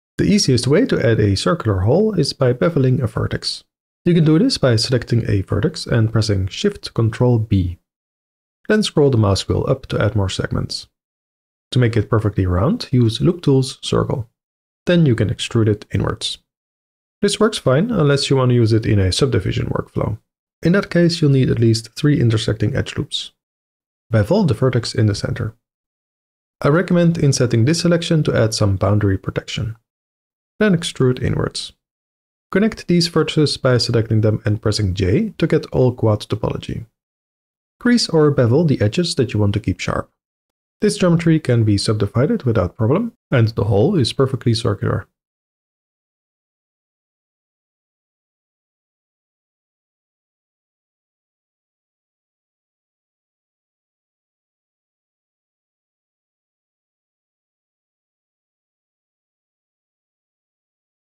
Easy Holes with Beveled Vertices | Blender Secrets
Do you want to make easy holes, that can also work in a Subdivision workflow?